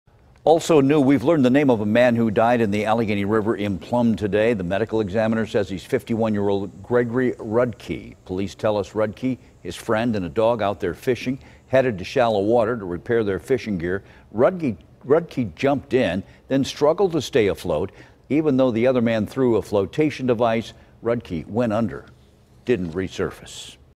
Man Dies After Jumping Into River While Fishing With Friend
A body was pulled from the Allegheny River in Plum on Saturday.